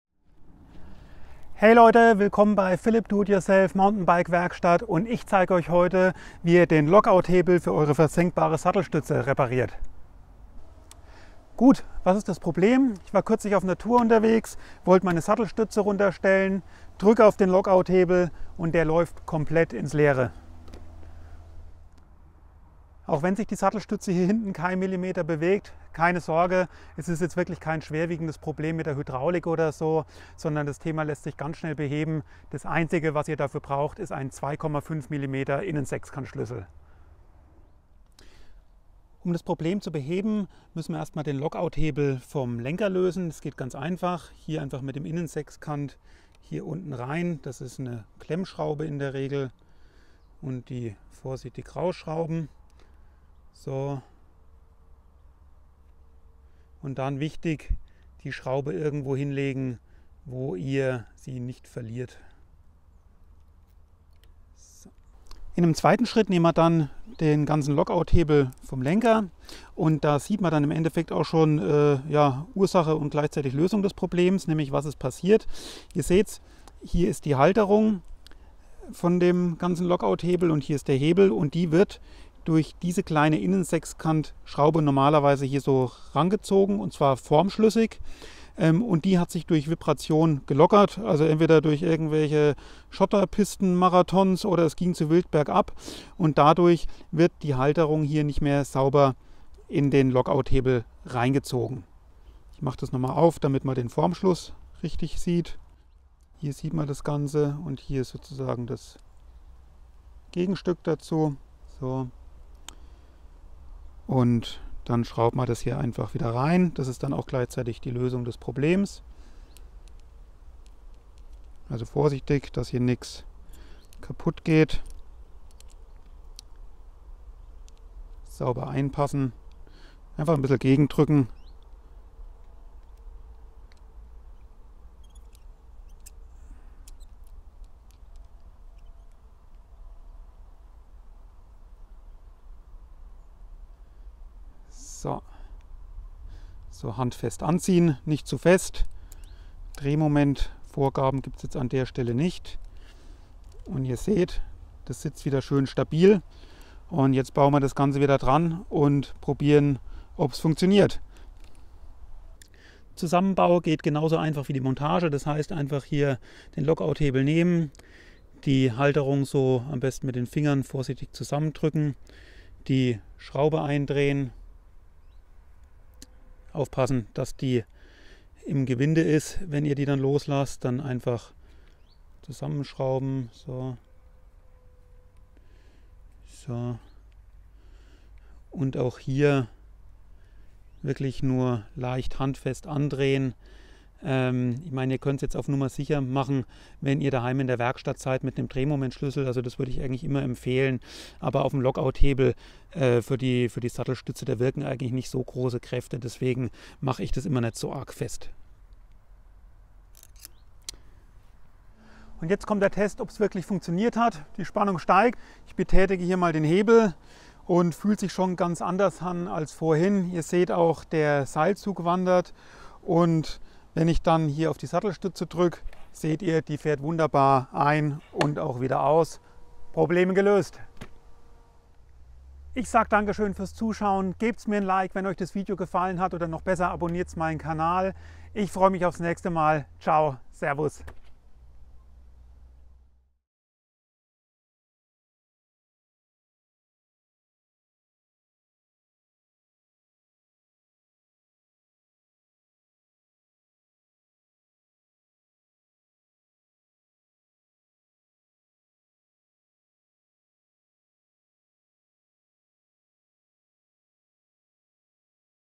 Reparatur Remote Hebel - Versenkbare Sattelstütze
Der Remote Hebel an der Dropperpost läuft komplett ins Leere und nichts passiert? Kein Problem!

In diesem Video zeige ich euch am Beispiel einer Fox Factory Transfer, wie ihr den Lockout Hebel für eure versenkbare Sattelstütze in ein paar wenigen Schritten repariert.

Das Schöne ist: das Ganze dauert nur ein paar wenige Minuten und ihr benötigt nur einen einfachen 2,5 Innensechskantschlüssel, heißt: ihr könnt das Ganze ohne großen Aufwand kurz vor oder auch während einer Tour wieder auf Stand bringen.

Hier noch eine Liste mit den gezeigten Produkten:

Viel Spaß beim Zuschauen, viel Erfolg und natürlich viel Spaß beim Biken!

Euer Philip

P.S. Schaut gerne auch in meine anderen Videos rein – hier findest sich bestimmt noch das ein oder andere Nützliche.